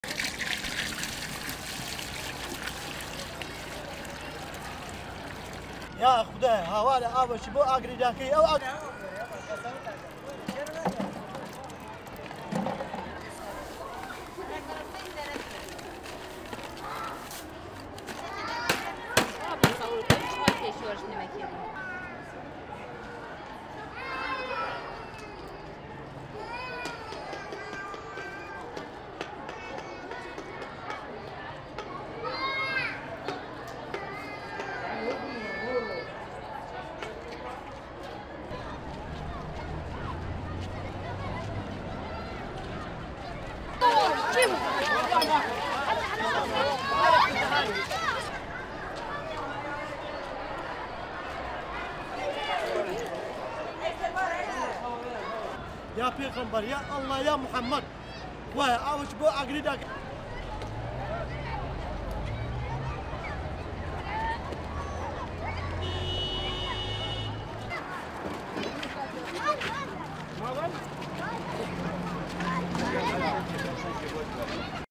IRAQIS FLEE VIOLENT OFFENSE OF ISLAMIC STATE
Iraqis flee violence as Islamic State mounts major offensive.